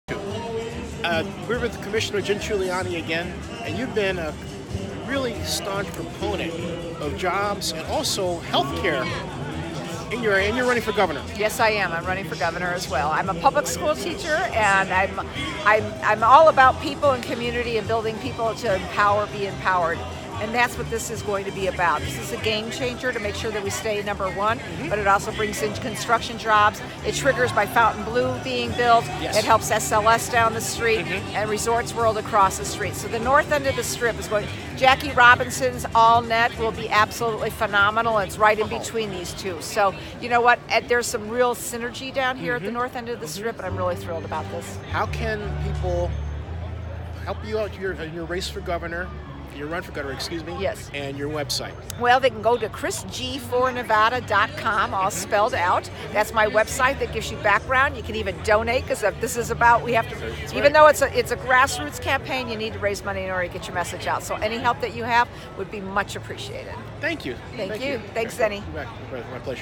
Chris Giunchigliani Clark County Commissioner On Her Run For Governor Of Nevada
Zennie62 Chris Giunchigliani Clark County Commissioner On Her Run For Governor Of Nevada CES2018 - (Zennie62 YouTube vlog) Maria Ayerdi Kaplan